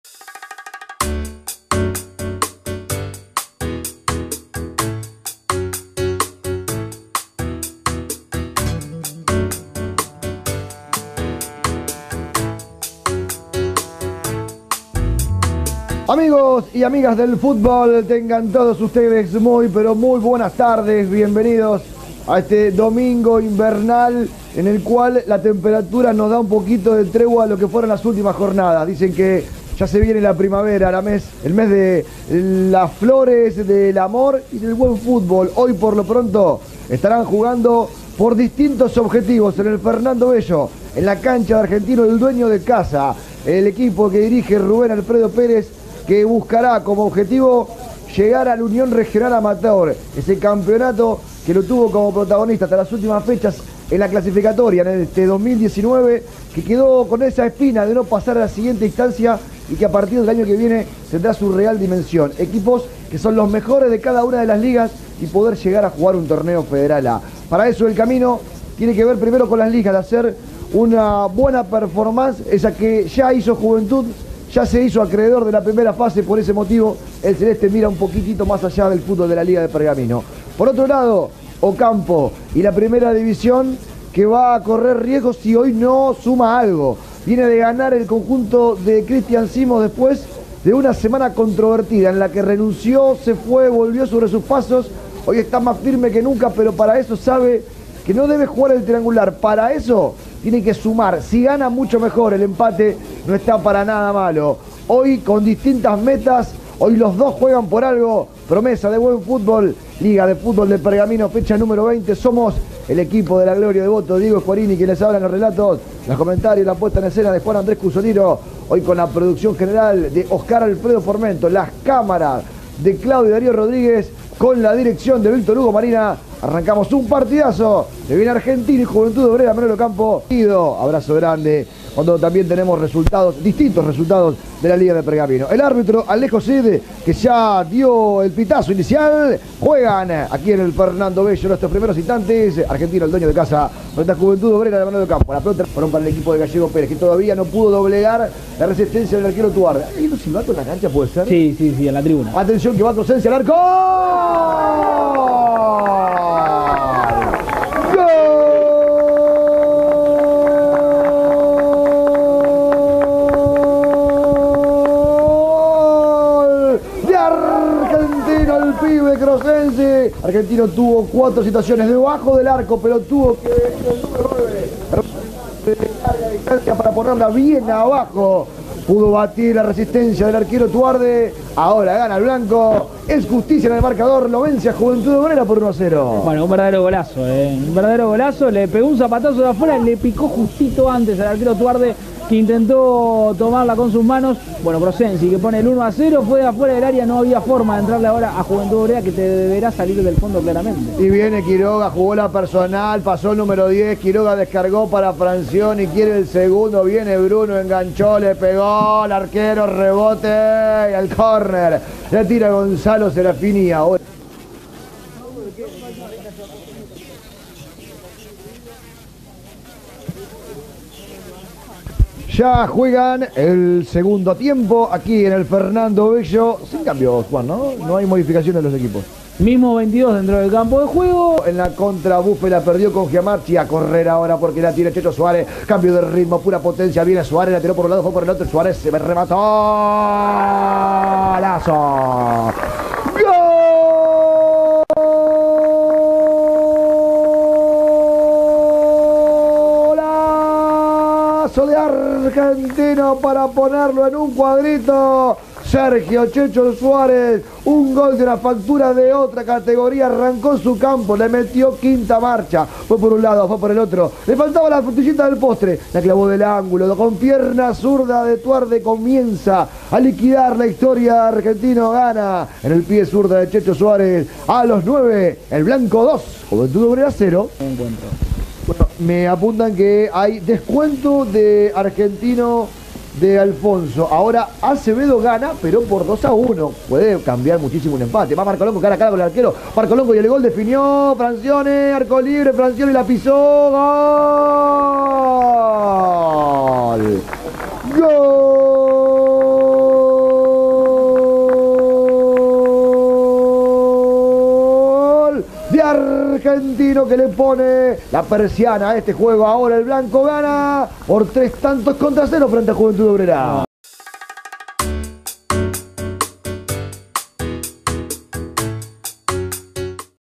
ARGENTINO 3 JUVENTUD OBRERA (Manuel Ocampo) 0- Fecha 20- LIGA PERGAMINENSE DE FÚTBOL 2019
ARGENTINO VS JUVENTUD OBRERA (Manuel Ocampo) -Fecha 20
LIGA PERGAMINENSE DE FÚTBOL
Una Producción de la Gloria o Devoto